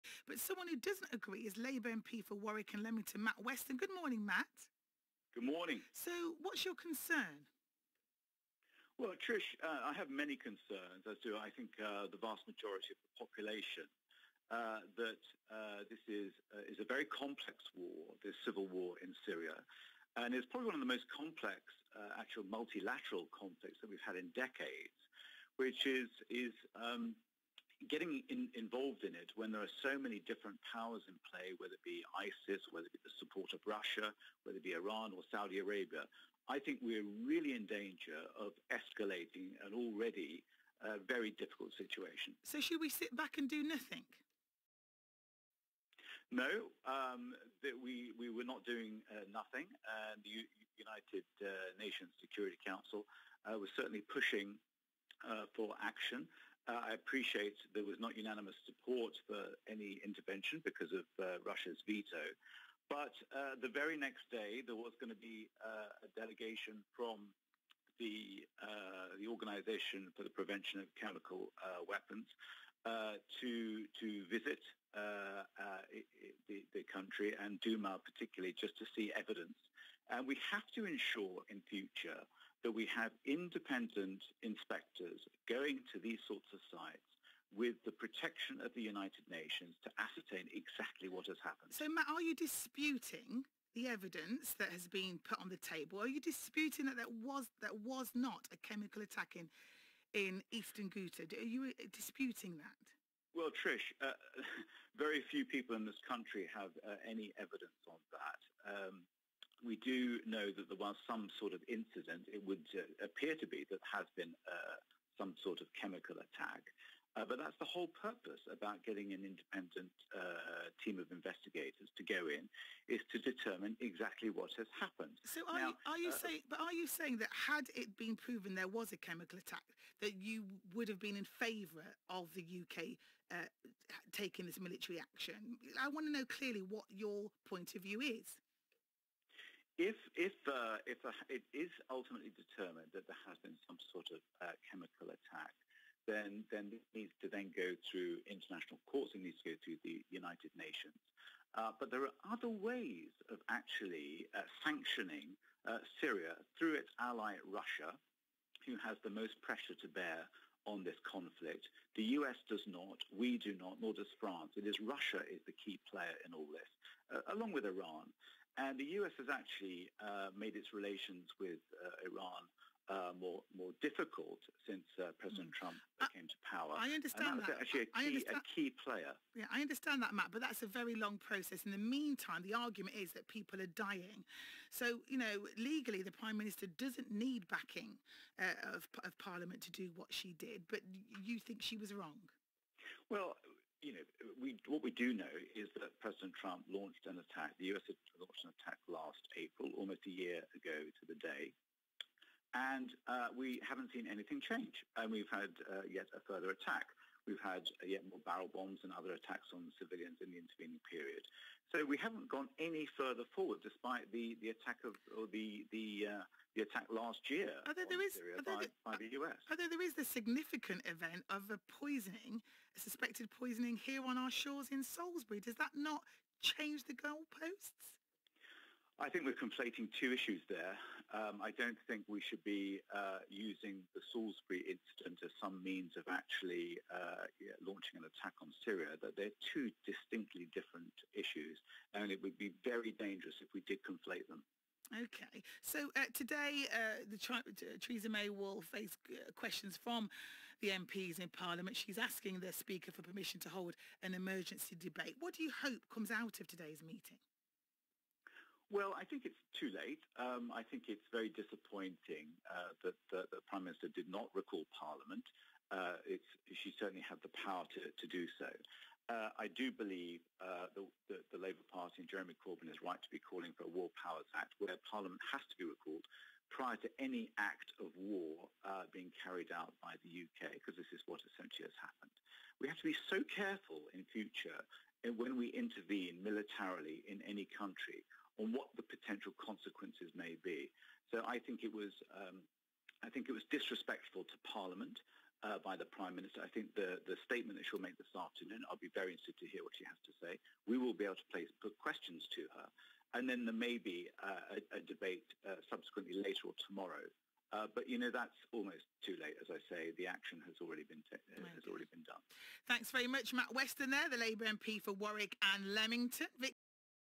Syria Airstrikes: BBC Coventry and Warwickshire
16/04/18: This morning I joined Trish Adudu on BBC Coventry and Warwickshire to discuss my opposition to the Government's recent airstrikes on Syria, without consulting Parliament. We need to let the OPCW complete their investigations. The Syria conflict is a complex, multilateral conflict with many powerful military groupings and the so UK should be extremely cautious about such interventions.